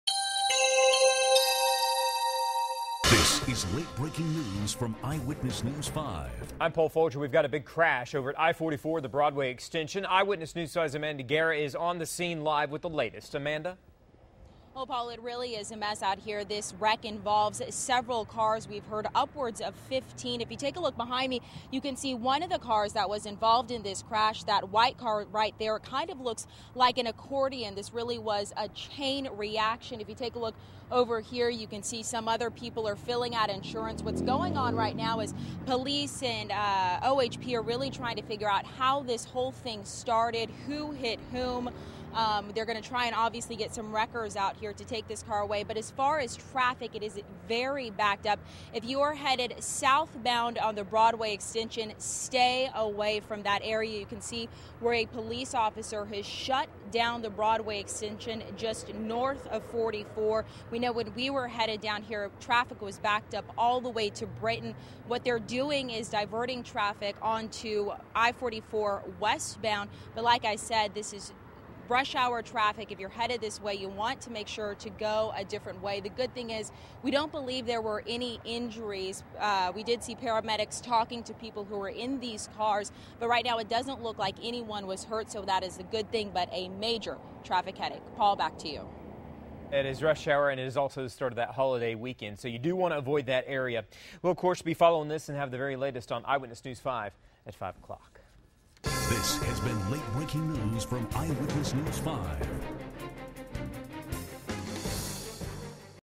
Part Of I-235 Closed Due To Crash
Part of the Broadway Extension is shut down Friday afternoon because of a multiple-vehicle crash, fire officials said.